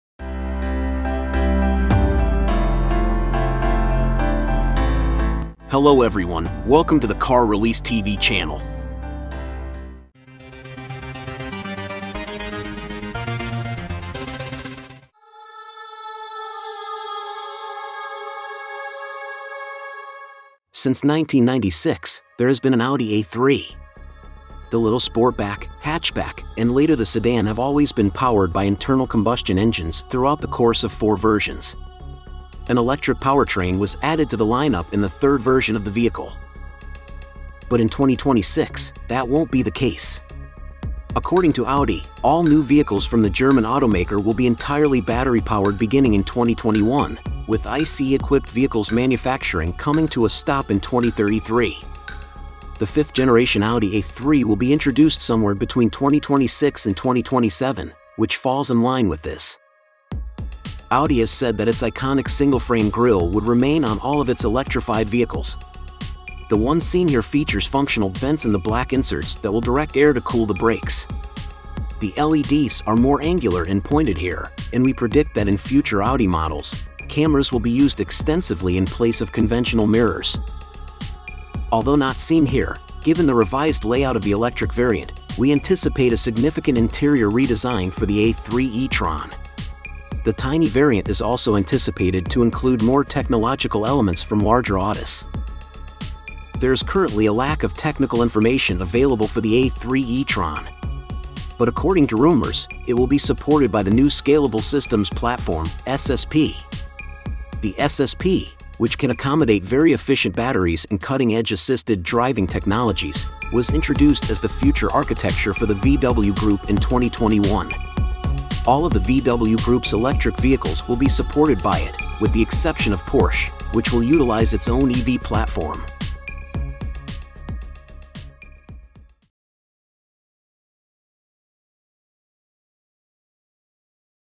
2023-2024 Audi A3 E-Tron Rendering - New Information | Coming Soon!!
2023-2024 Audi A3 E-Tron  - Audi has said that its iconic single-frame grille would remain on all of its electrified vehicles. The one seen here features functional vents in the black inserts that will direct air to cool the brakes. The LEDs are more angular and pointed here, and we predict that in future Audi models, cameras will be used extensively in place of conventional mirrors.